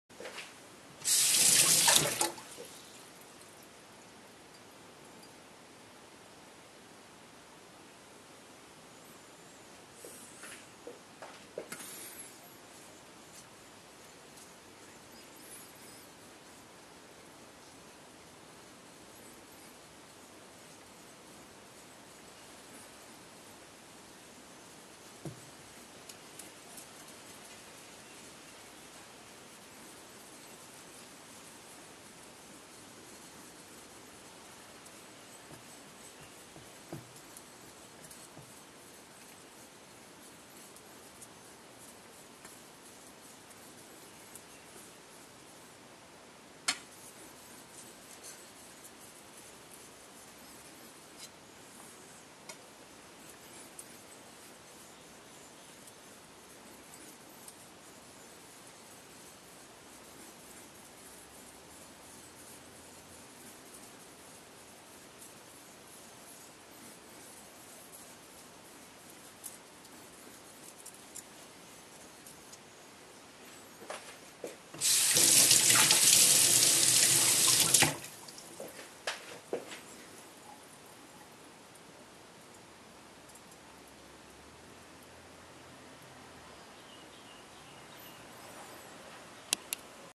Yaginoshima Suita 3lb. 7oz. Japanese Natural sharpening stone
It is not too hard not too soft which is very nice.

In my scale (1-10),
the hardness: 8.5
the particle size: 8.5